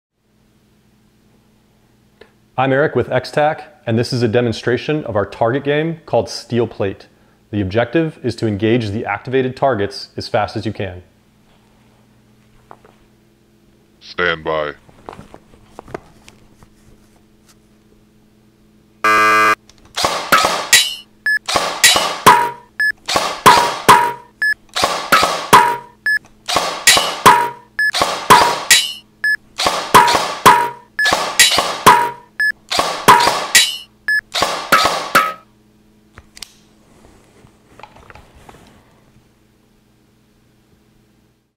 Steel Plate Drill with XTAC
Steel Plate game demonstration with Xtac Pro.  Engage the activated targets as rapidly and accurately as possible.

Tactical Training, Competition, and Games
Firearms Training and Safety
Home Shooting Range
Force-On-Force